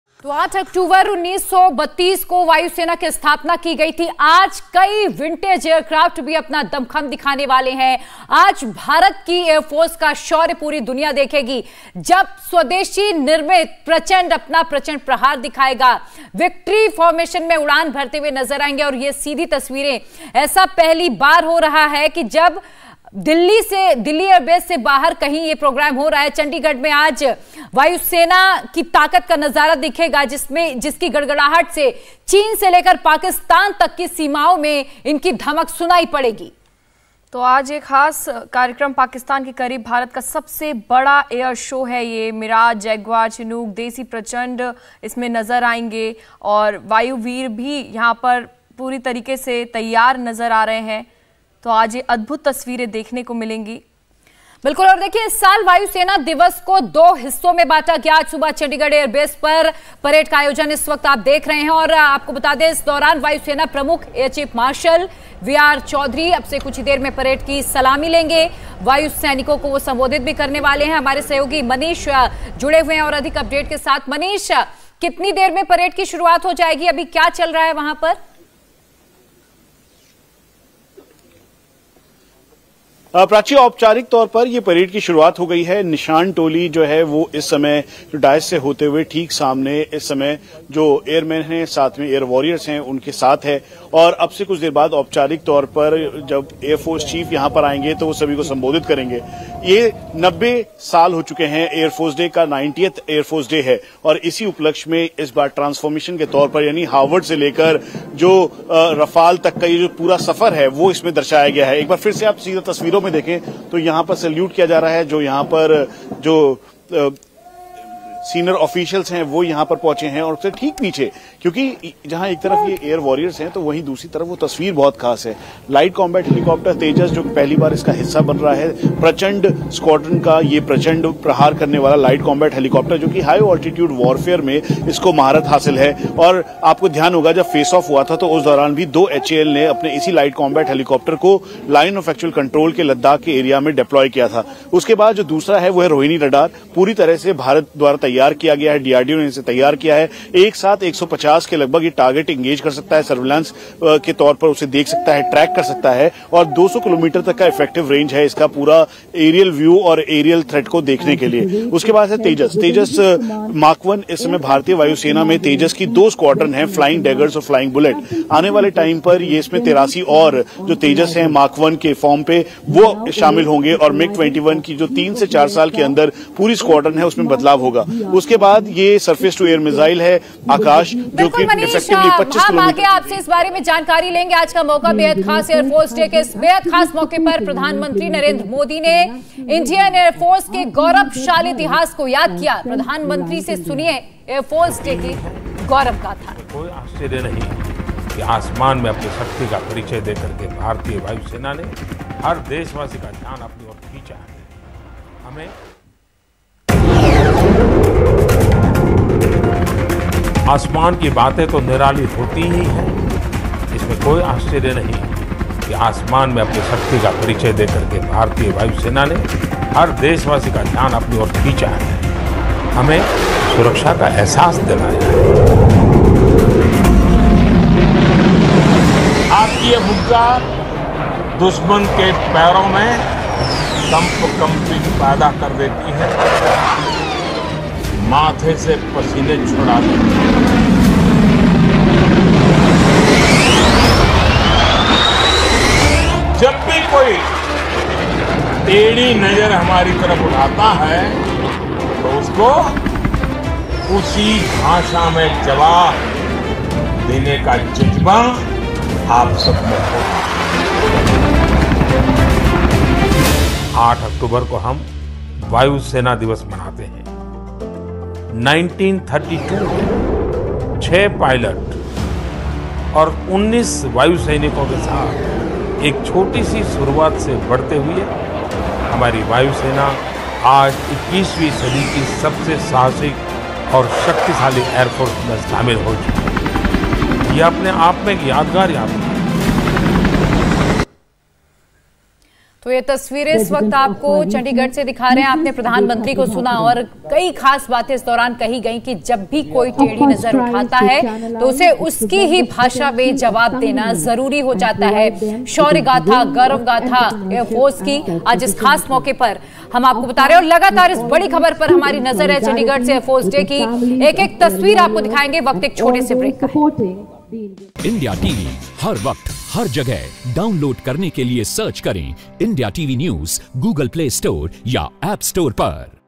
Indian Air Force Day 2022: Chandigarh के आसमान में वायुसेना करेगी बड़ा एयर शो | Chandigarh Air Show
Indian Air Force Day 2022: इंडियन एयर फोर्स दुनिया की चौथी सबसे ताकतवर वायुसेना है जो अपना 90वां स्थापना दिवस मनाने की तैयारी में जुटी है.. 8 अक्टूबर एयरफोर्स डे से पहले आज चंडीगढ़ में फुल ड्रेस रिहर्सल के जरिए जवानों ने एयरफोर्स के जवानों ने अपना दमखम दिखाया.